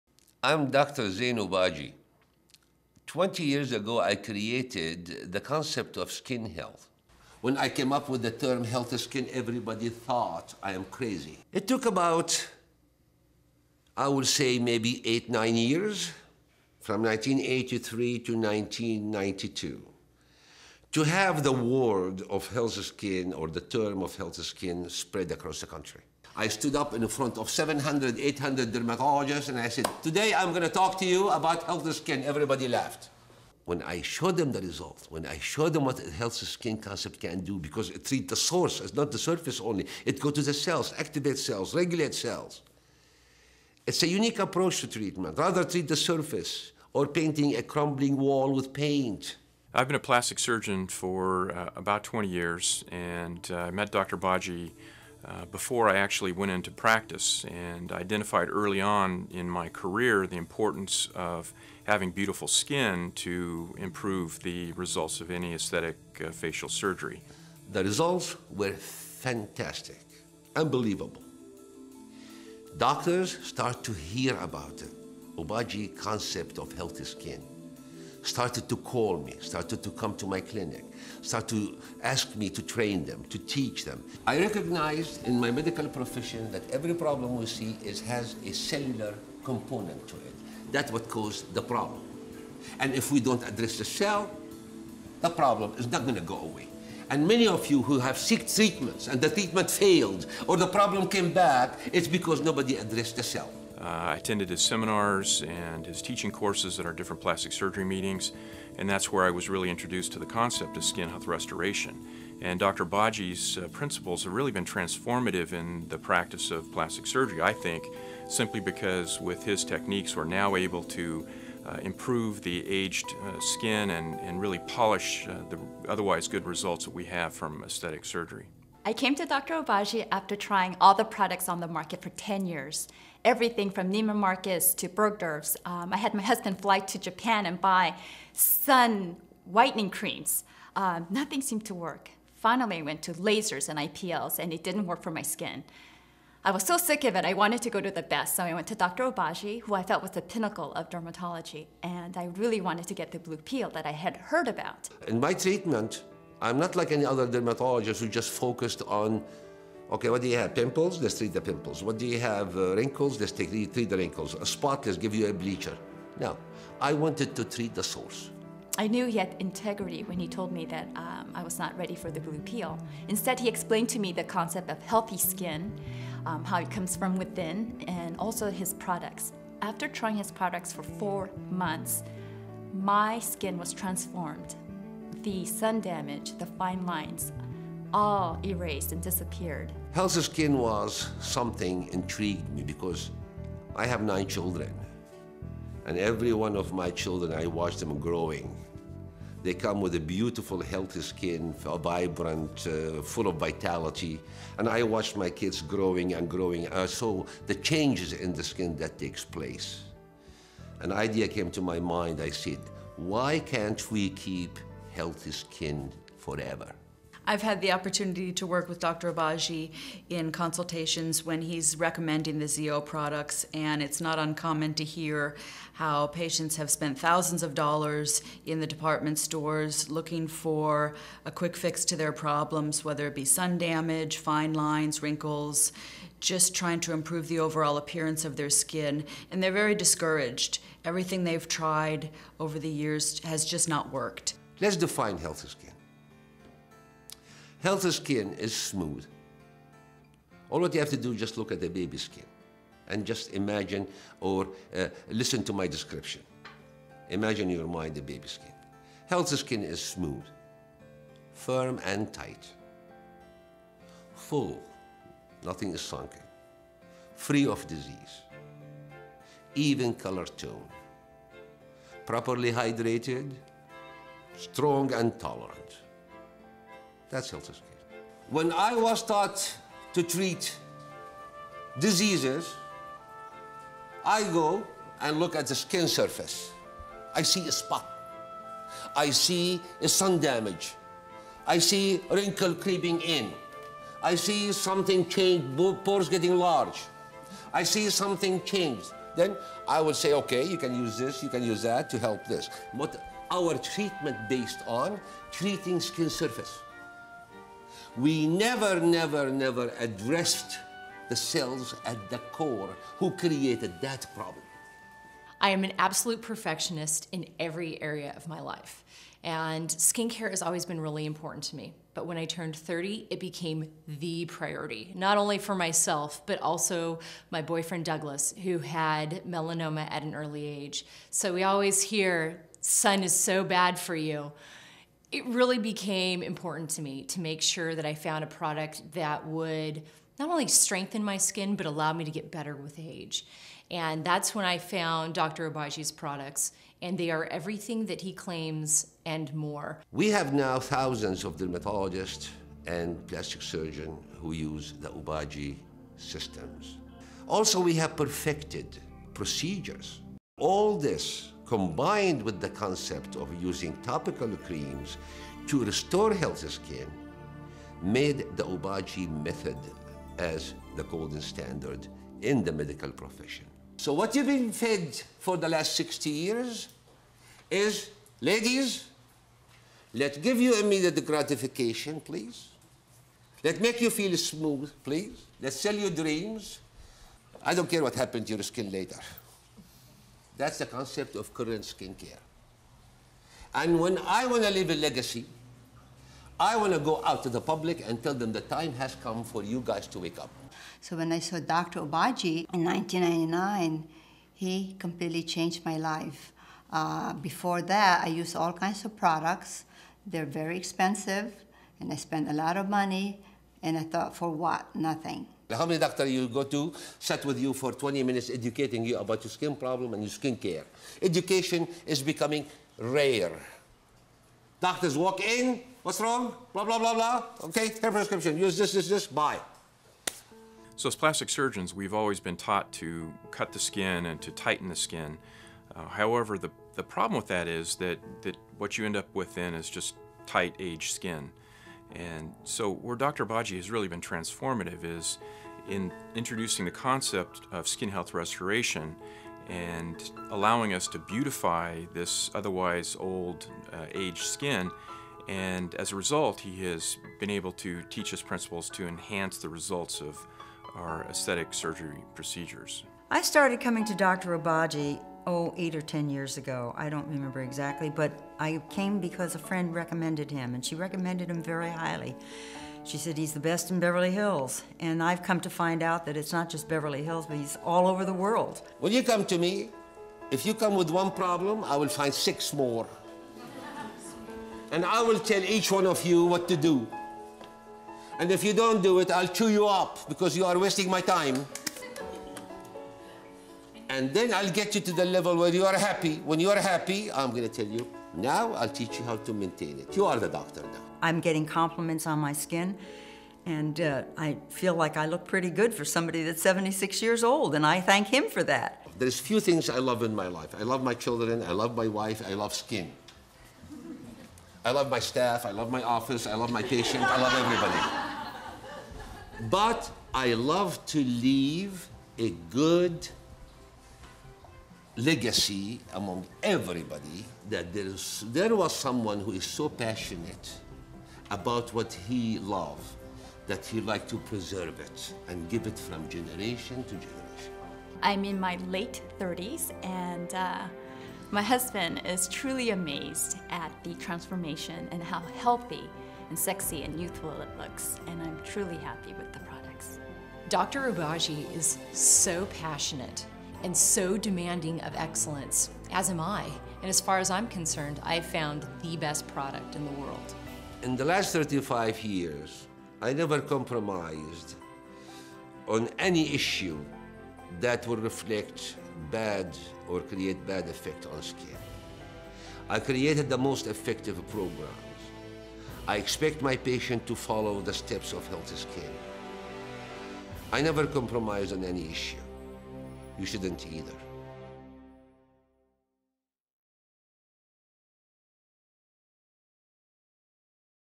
Dr. Zein Obagi Speaks on Skin Health
Dr. Obagi is the ZO Skin Health, Inc. medical director and is responsible for the development of new skincare treatments, protocols and product to achieve healthy skin. Additionally, he is a board certified practicing dermatologist in Beverly Hills, California, and an author and educator that has presented more than 200 skin health lectures throughout the world. For more than 35 years Dr. Obagi has defined and continues to advance skincare to include the concept of creating and maintaining healthy skin as opposed to just treating disease and damaged skin.

Dr. Obagi originally conceived and brought to market what would become the world's most recognized brands of physician dispensed skincare products -- the original Obagi Nu-Derm® and Obagi Blue Peel® kit. Now Dr. Obagi, as the ZO Skin Health medical director, has redefined the concept of skin health to include comprehensive solutions that bridge the gap between therapeutics and maintaining truly healthy skin.